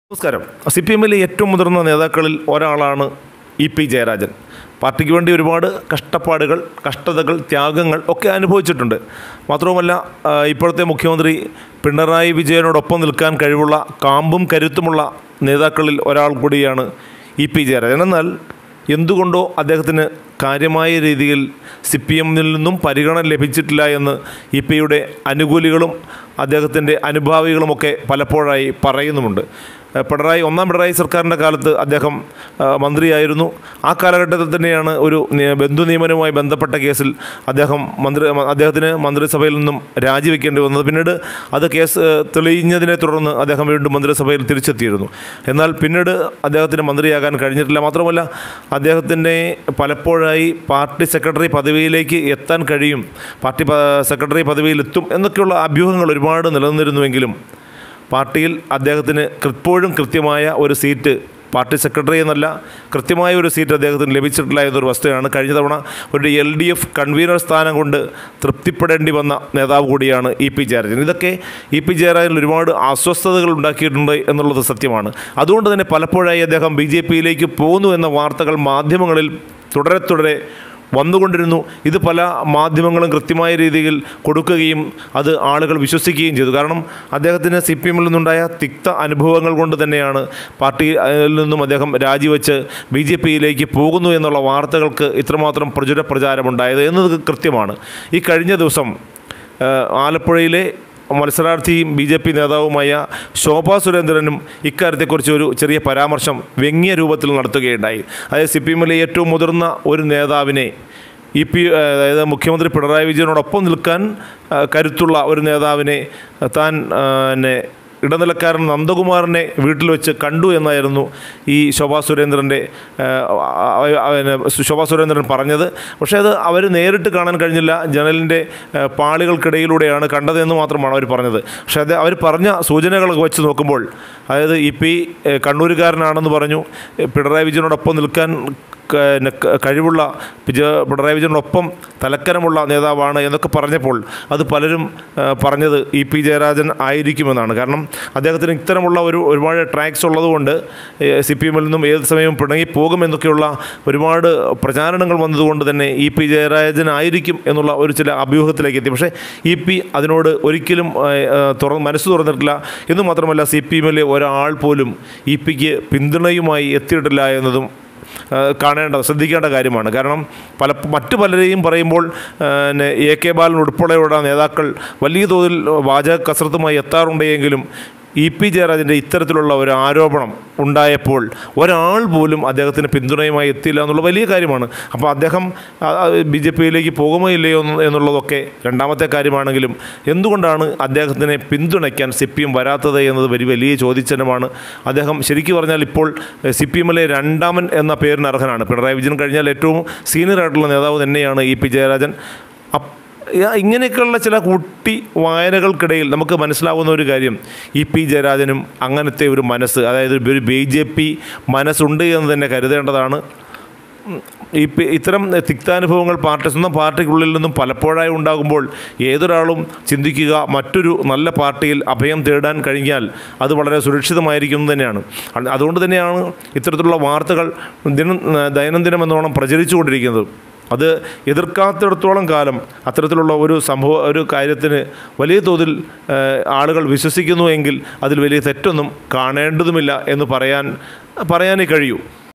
ഇ.പി.ജയരാജൻ ബി.ജെ.പിയിലേക്ക്... ഒന്നും മിണ്ടാതെ പിണറായി !!
E P jayarajan\\